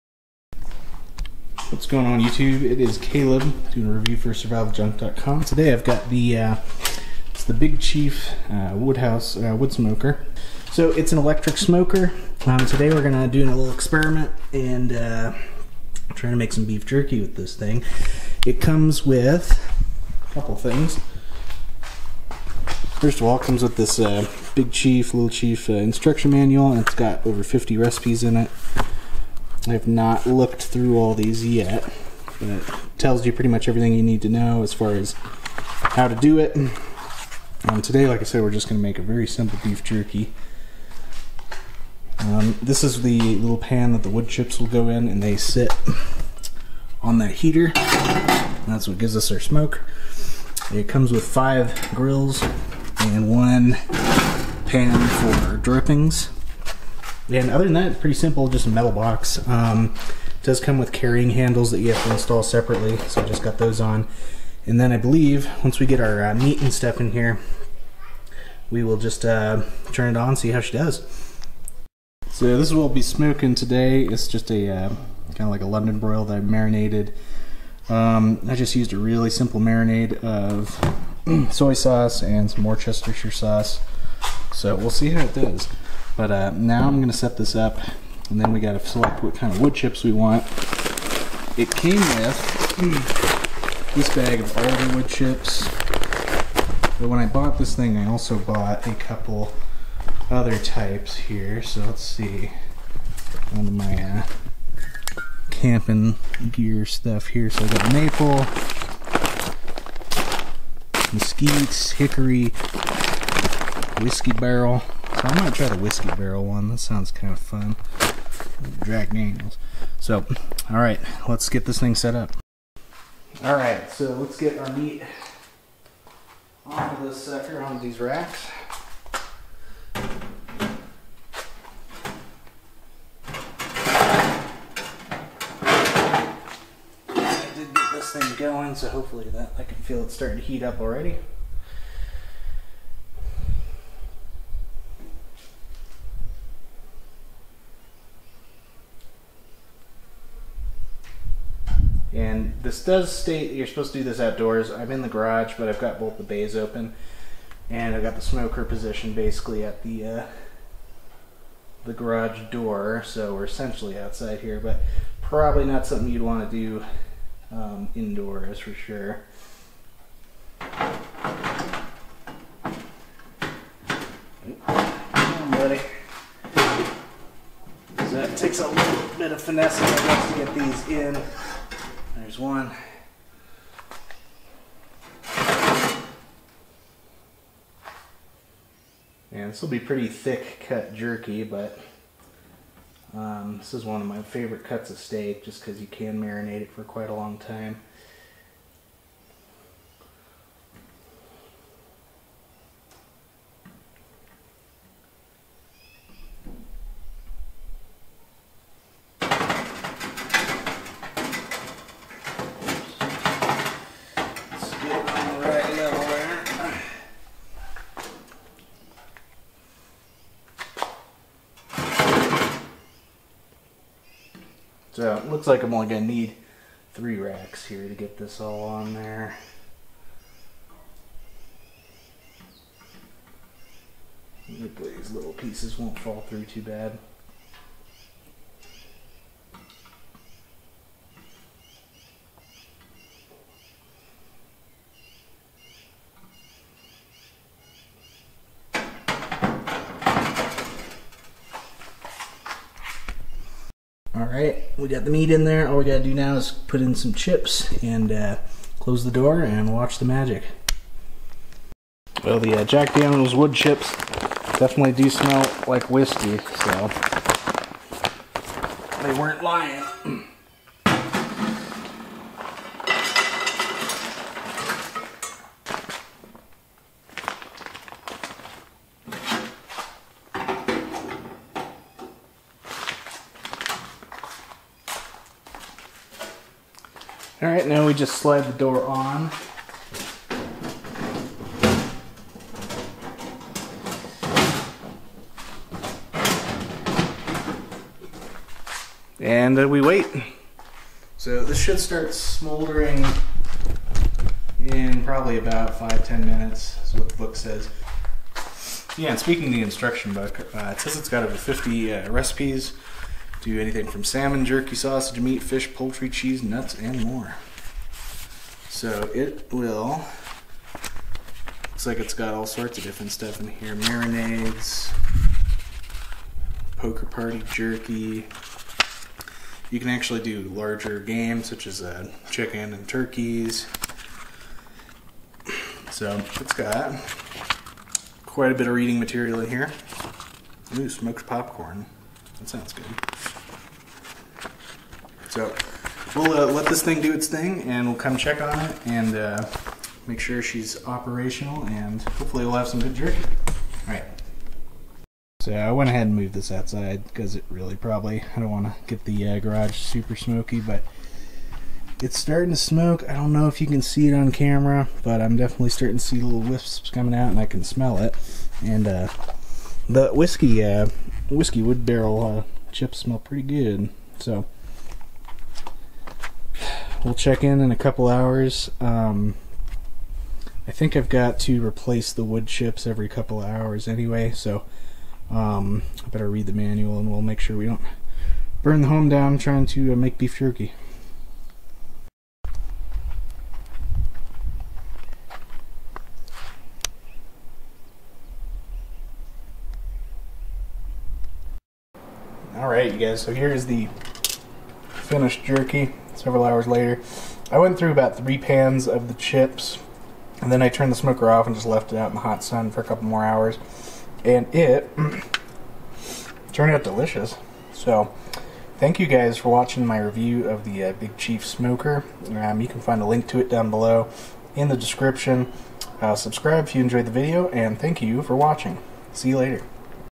Big Chief Electric Smoker by Smokehouse Products Review
Big Chief wood smoker!!  This is my first attempt at using a smoker to create beef Jerky and I really enjoyed using the Big Chief!  Super simple, it runs on regular 120 v plug in and just set the chips, and let er' smoke!   This product is for OUTDOOR use only, and you'll see why once the wood chips start smoking.  I used three batches of chips for this beef jerky, and then unplugged the unit and allowed the meat to dry for a few more hours in the smokehouse in the sun.   Great item to have if you're interested in creating smoked foods that help to preserve and last longer.  The jerky turned out great with a sweet smokey flavor with just a hint of whiskey.

🏡Got a Colorado real estate question?  Message me! 🏘️